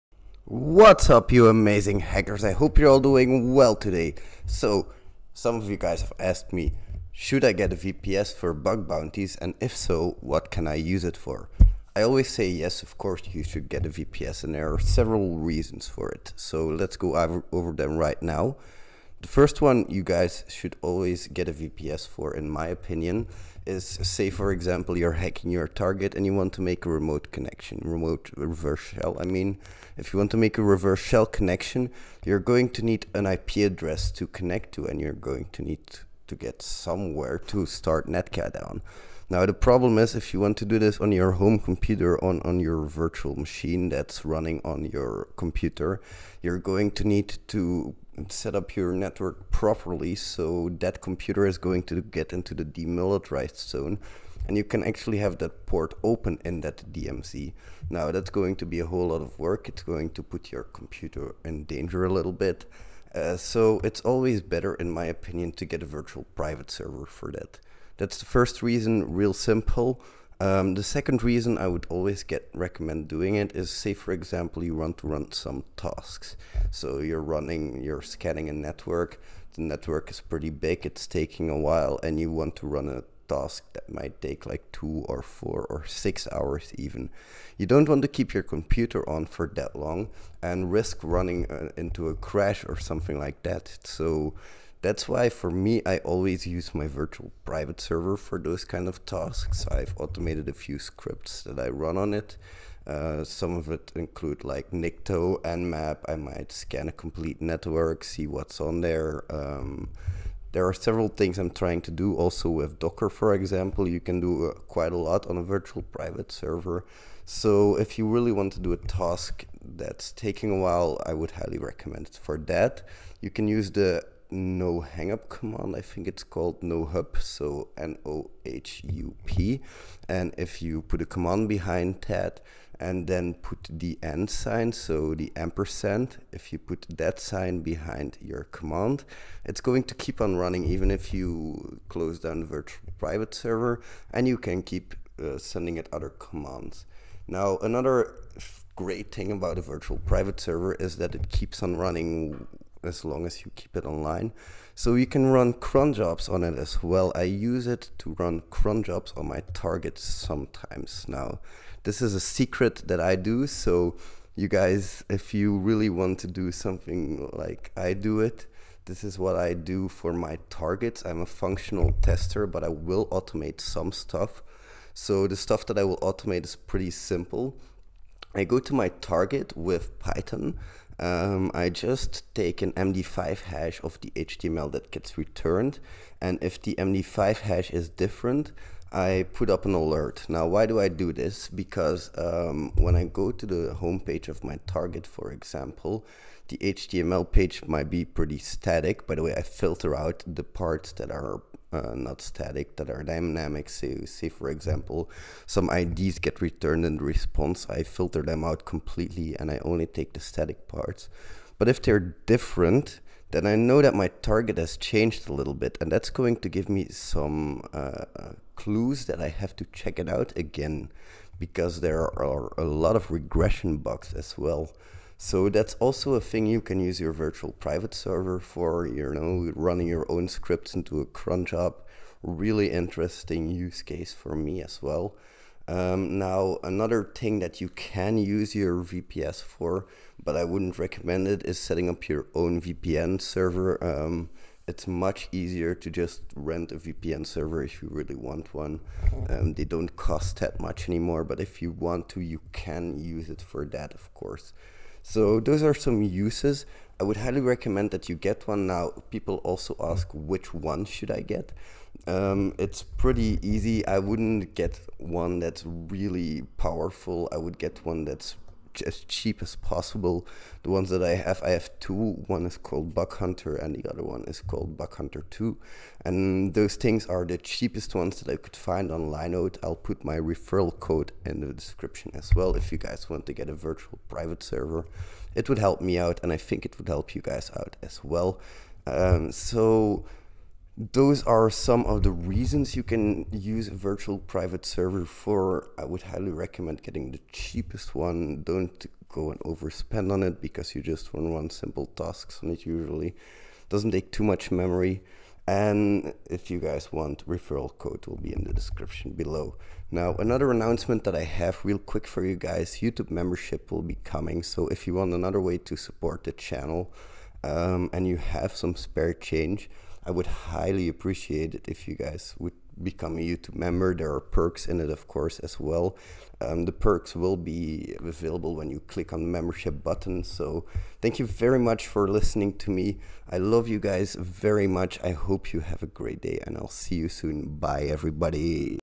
Should i get a VPS
Virtual private servers are very useful in bug bounty's, let's explore some use cases and go over which VPN to get.

thexssrat